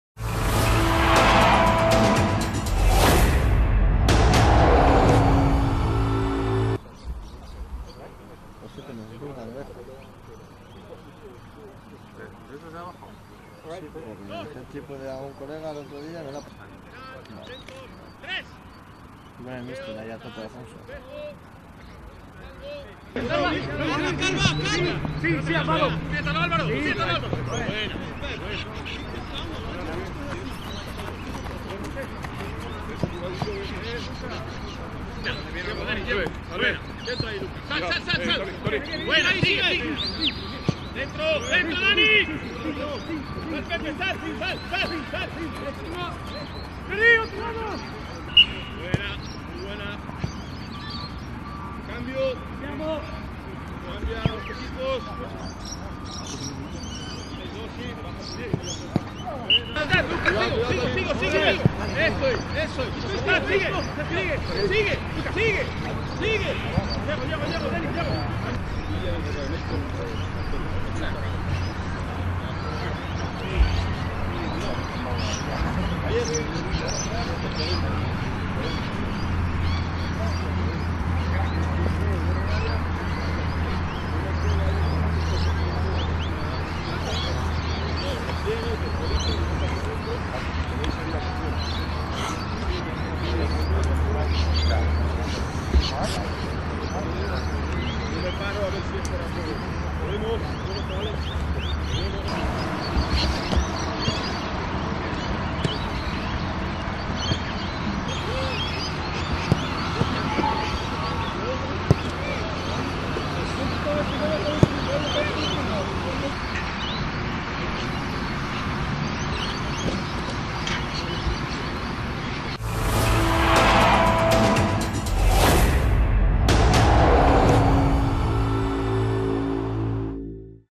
Rafa Benitez takes charge of Real Madrid...
Real Madrid's new manager Rafa Benitez takes charge of his superstar squad as they prepare for their pre-season tour.